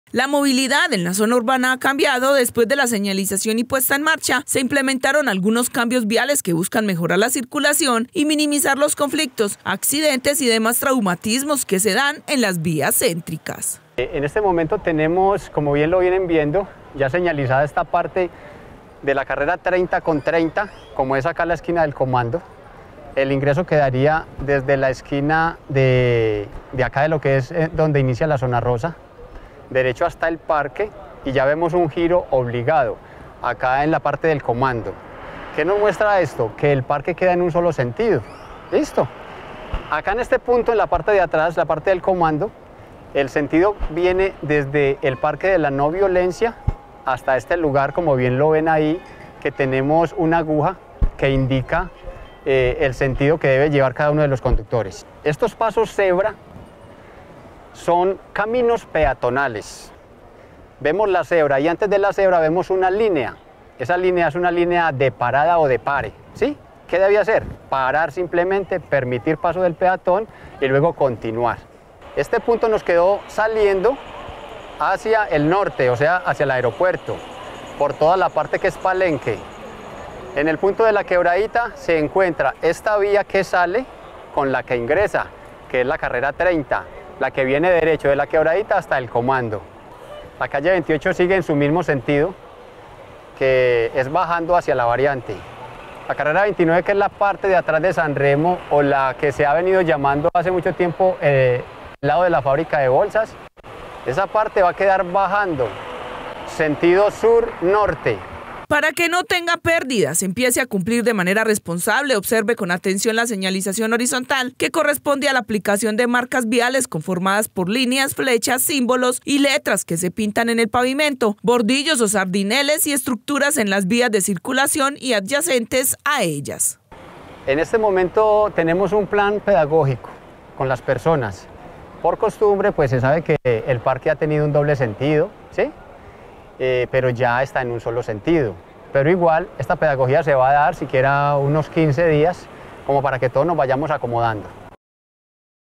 MOVILIDAD DEL PARQUE EN UN SOLO SENTIDO
Después de hacer efectiva la nueva señalización y el cambio de movilidad por el parque principal que será en un solo sentido, les mostramos de una forma pedagógica de cómo deberá transitarse por las vías aledañas, para que se evite llamados de atención, realizamos el siguiente informe